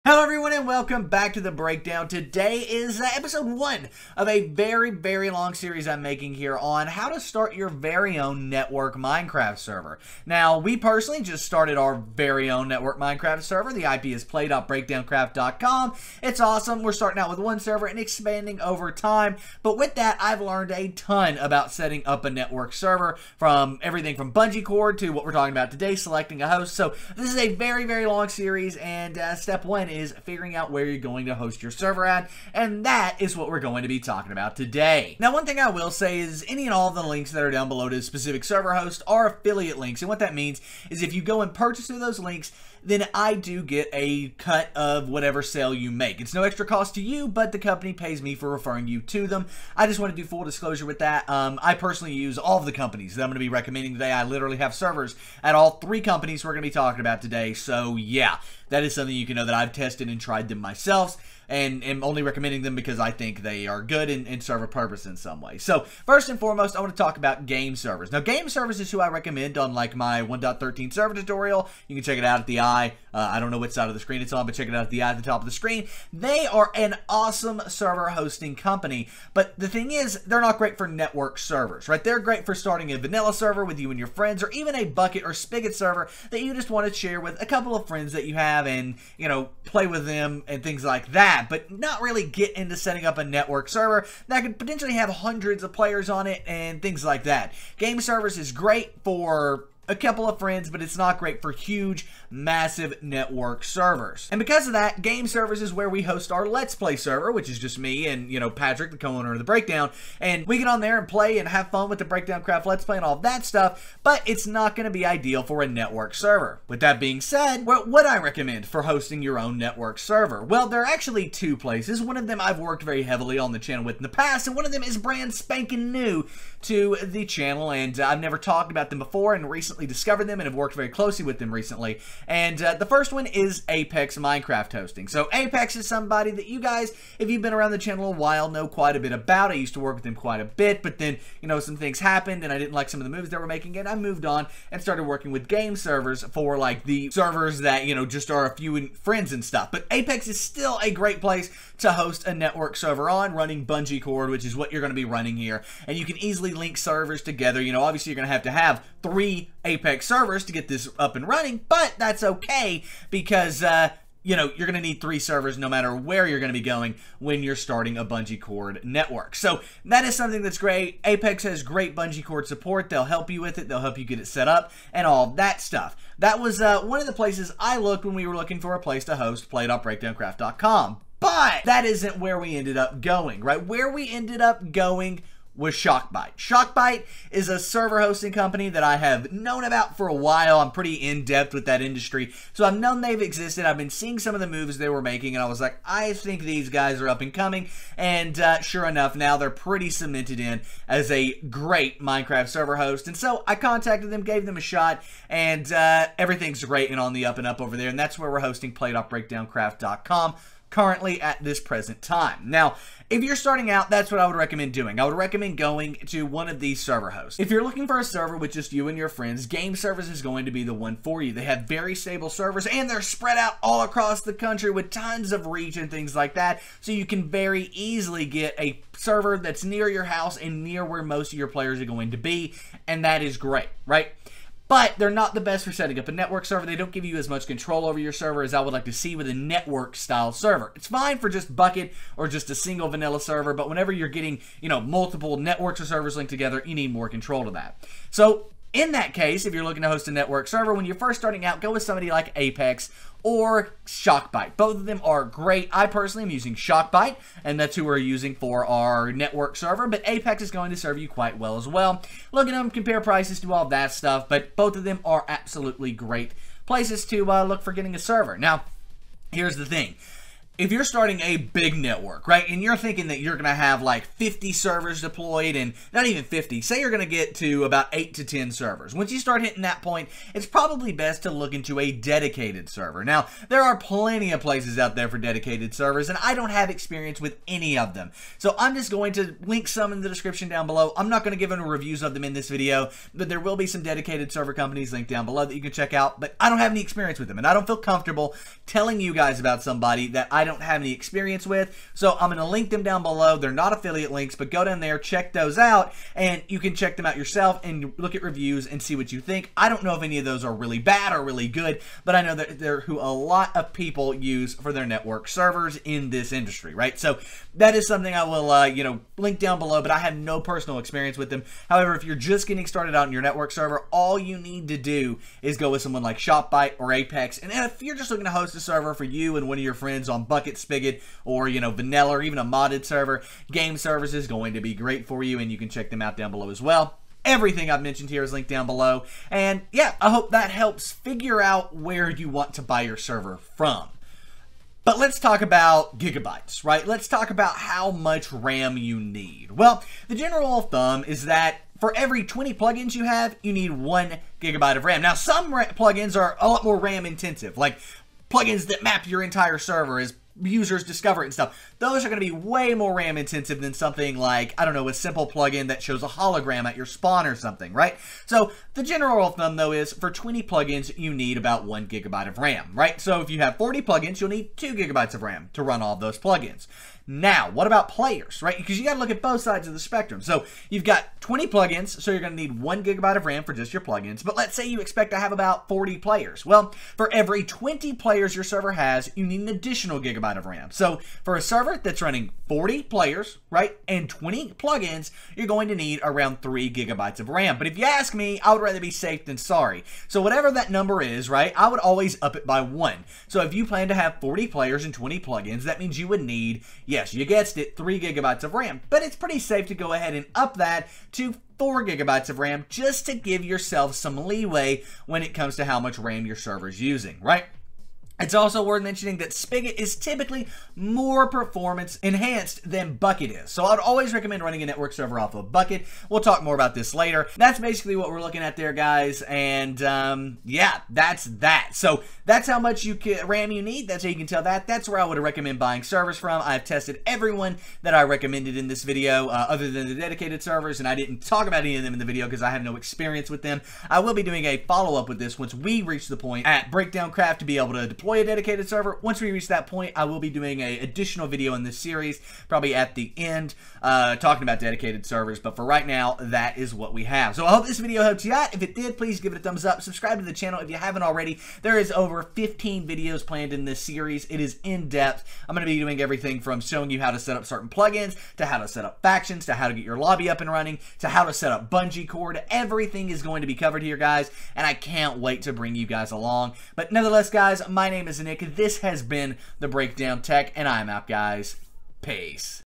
How To Select Your Minecraft Server Host - How To Make A Minecraft Server Network Ep. 1
How do you choose the right host for your Minecraft server? Well, I go over everything you need to know when choosing a host from what hosts I recommend to even how much RAM you need for a Minecraft server. Everything is covered here in part one of my series on how to make a Minecraft server network!

Start A Server For You & Your Friends!

Dedicated Server Options!

About this video: This video is my complete guide to choosing your Minecraft server host. There are tons of hosting companies out there from ApexMinecraftHosting to GameServers to MCProHosting, and that is just naming a few! How do you decide which one is the best for you? On top of that, how much RAM does your Minecraft server even need? Well, I go over that and more in this video.

This is actually where you need to start when you are starting your very own Minecraft 1.13.1 server network as, without a server host, you can't get anywhere! From where to need to start to how to move into dedicated servers, I cover literally every kind of Minecraft server you could possibly be looking for, and I make the best recommendations that I possibly can for a Minecraft server.

I also talk about the amount of RAM you need to start a Minecraft server. Basically, the general rule is that you need 1GB of RAM for every 20 players that you are going to have on your server. You are also going to need 1GB of RAM for every 20 plugins your server has. So, if you have 40 players with 20 plugins, you would need 3GB of RAM. I would suggest, however, add 1GB of RAM to that just to be safe, so 4GB of RAM should be able to run a 40 player, 20 plugin server without any issues!

And that is everything that you need to know about selecting a Minecraft server host in order to start your server network! It really helps me out, and it means a ton to me. Thank you very, very much in advance!